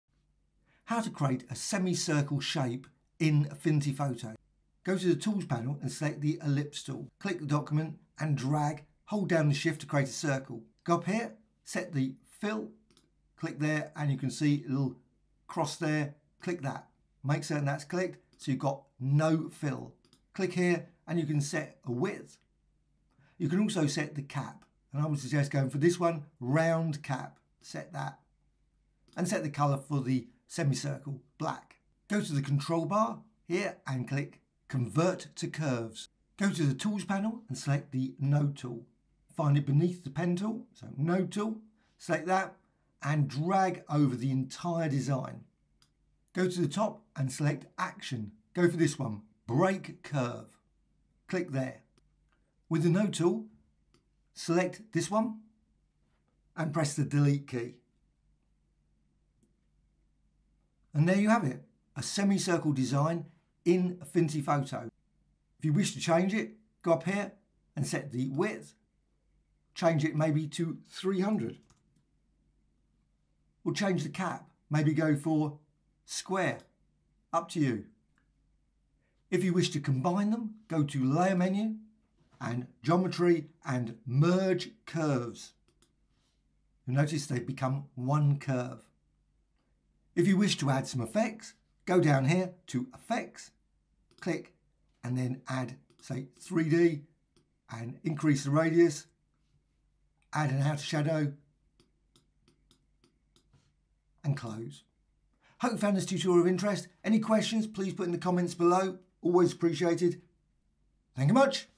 Affinity Photo : How To Create SEMI CIRCLE Shape | Tutorial | Half Circle Vector Design
How to create a semi circle / half a circle in Affinity Photo

How to create a semi-circle shape in affinity photo ???
Go to the tools panel and select the ellipse tool click the document and drag hold down the shift to create a circle go up here set the fill click there and you can see it'll cross there click that make certain that's clicked so you've got no fill click here and you can set a width you can also set the cap and I would suggest going for this one, round cap set that and set the color for the semi-circle black go to the control bar here and click convert to curves go to the tools panel and select the no tool find it beneath the pen tool so no tool select that and drag over the entire design go to the top and select action go for this one break curve click there with the no tool select this one and press the delete key

and there you have it a semicircle design in Affinity Photo if you wish to change it go up here and set the width change it maybe to 300. we'll change the cap maybe go for square up to you if you wish to combine them go to layer menu and geometry and merge curves you'll notice they've become one curve if you wish to add some effects go down here to effects click and then add say 3d and increase the radius add an outer shadow

and close hope found this tutorial of interest any questions please put in the comments below always appreciated thank you much